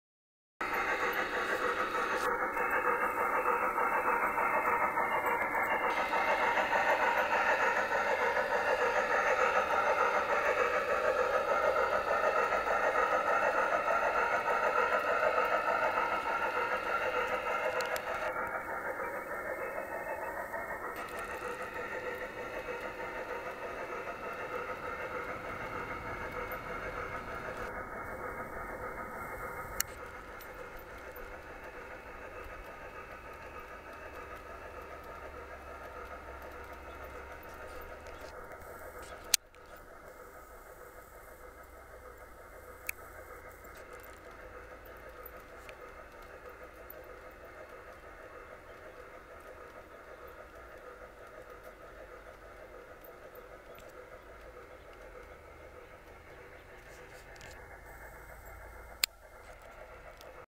K 27 smoke
Bachman K 27 with Rodney Eddington Gear box,Harbor Hobby smoke unit ,sierra sound  completely  gutted and hard wired 2 revo control units( motor+ smoke )